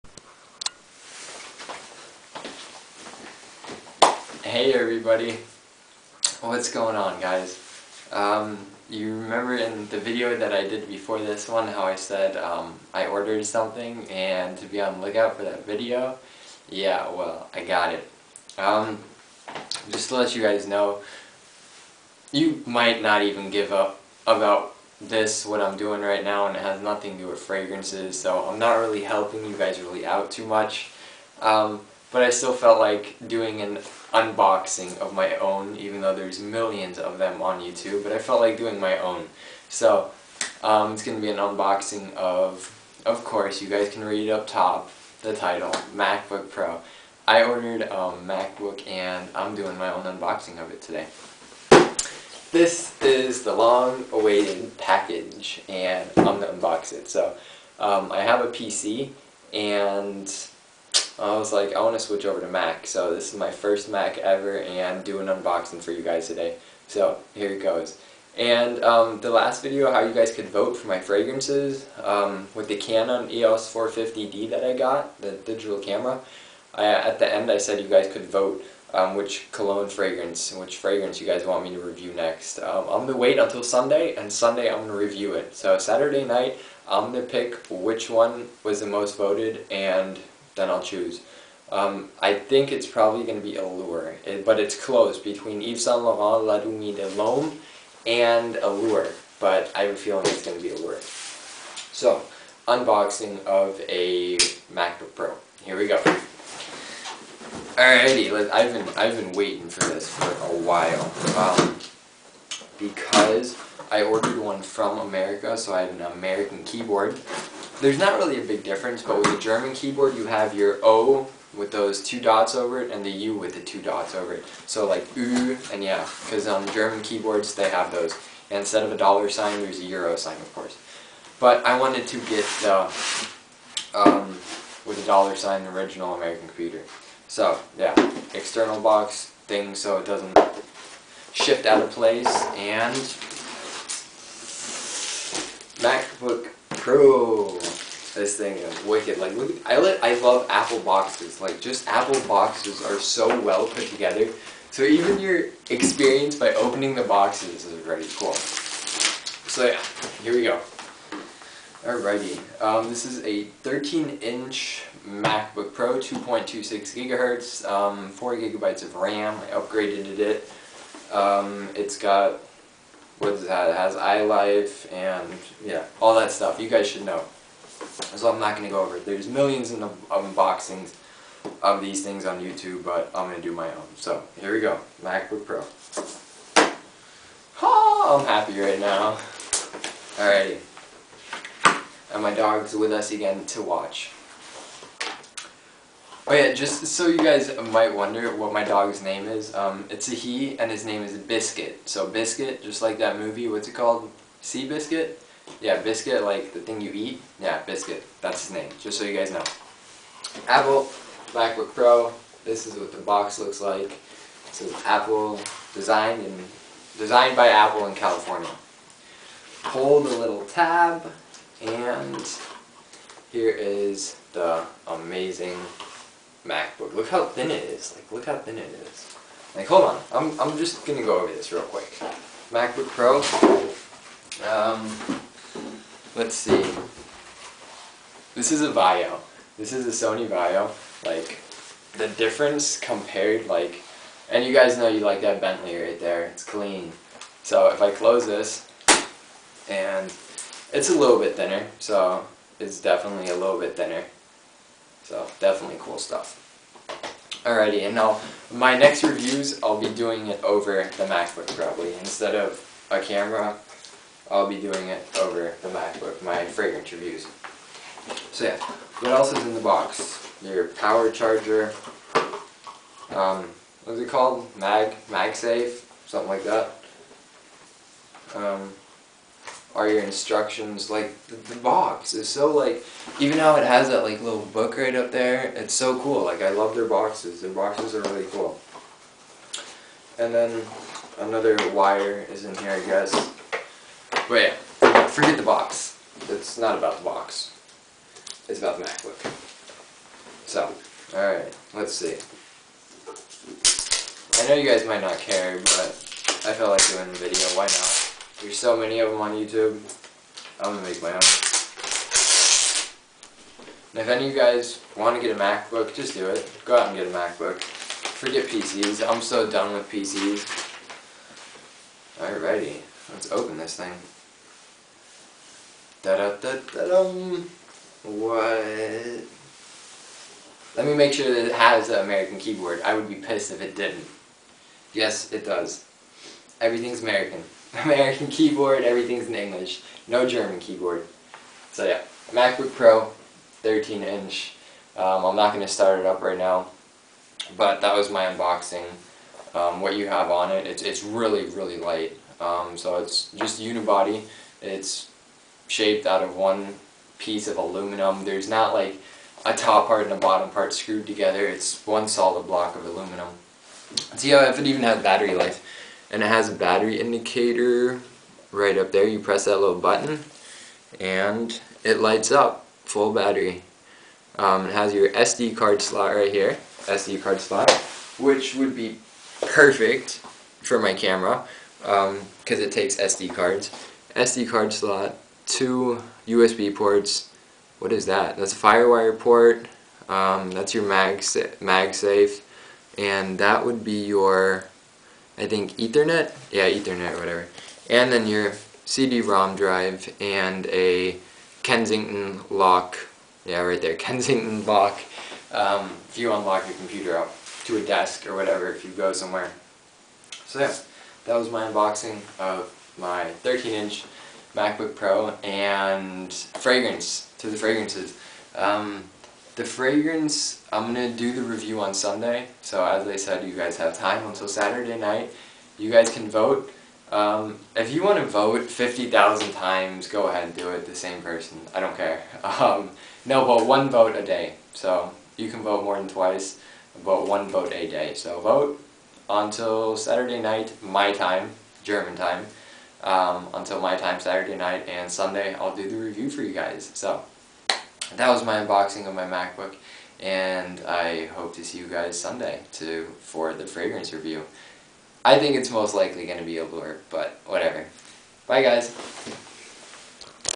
Macbook Pro unboxing
finally got my package :) macbook Pro 13 inch / i know you guys probably dont really care but i still felt like doing my own unboxing